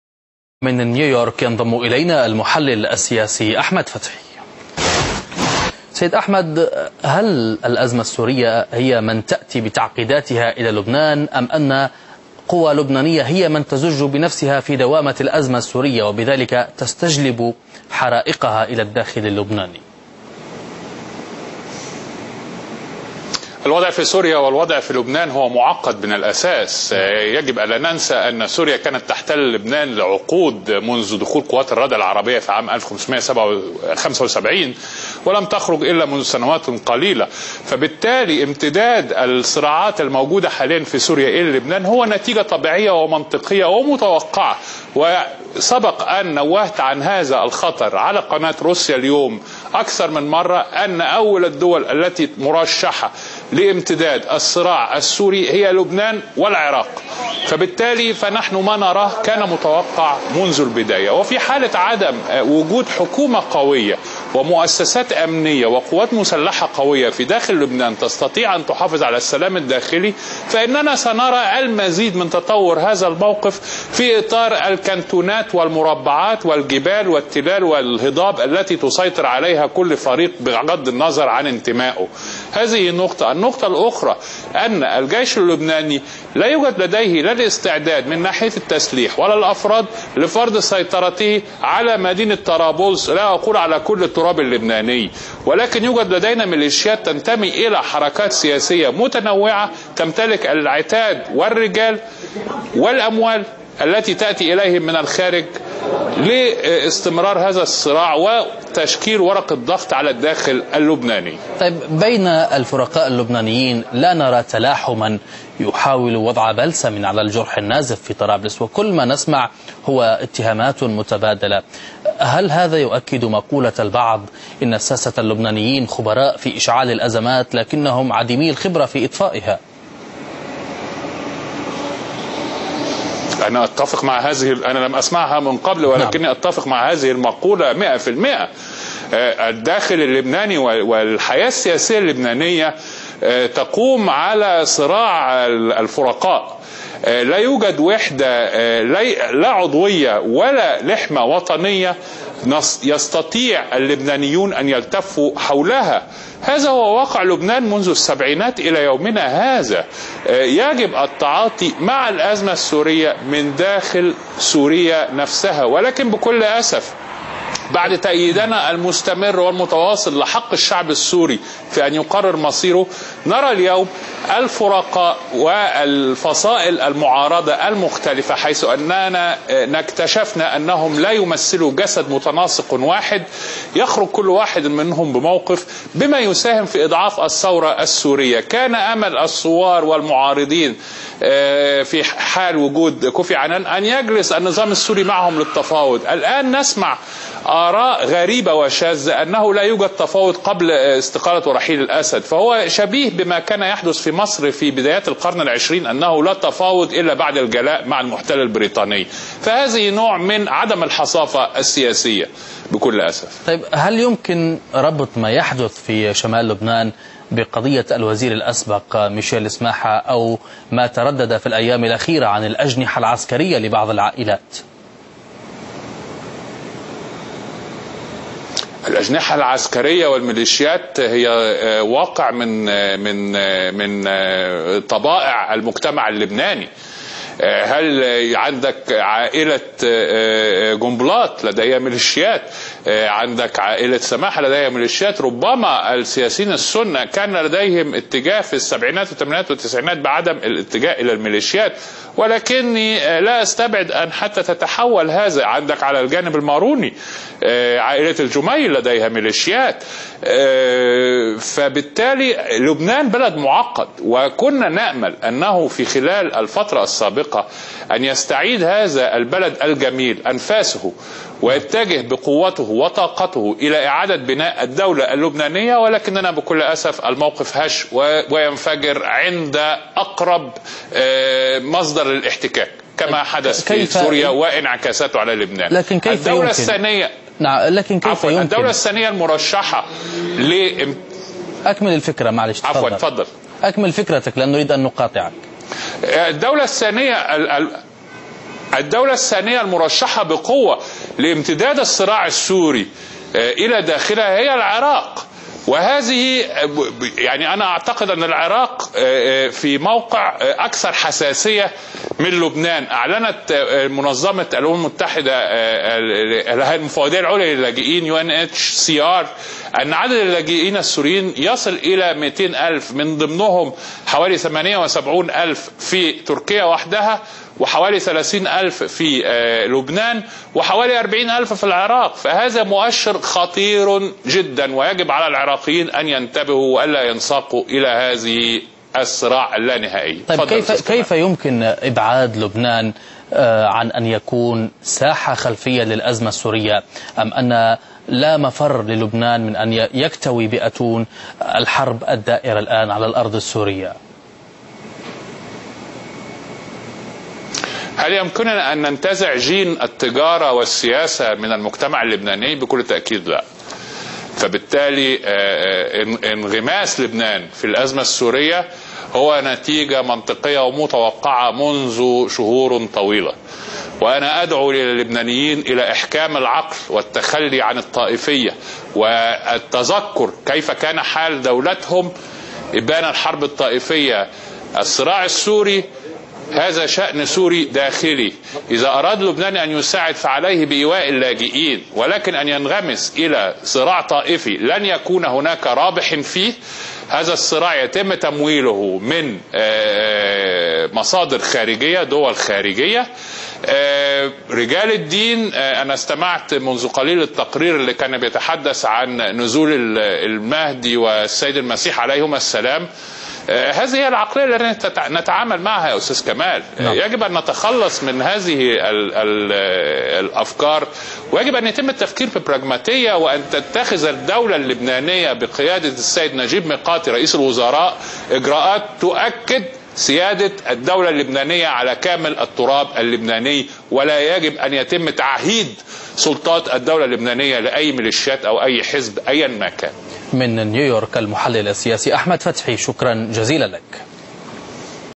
انزلاق لبنان و قريبا العراق إلي المستنقع السوري
في حديث لقناة "روسيا اليوم" من نيويورك أعرب المحلل السياسي أحمد فتحي عن اعتقاده بأن أول الدول المرشحة لامتداد الصراع السوري هي لبنان والعراق، مشيرا إلى أنه يرى وضع العراق حتى أكثر حساسية من لبنان. وأضاف أن ما يحدث حاليا في شمال لبنان كان متوقعا منذ بداية الأزمة السورية.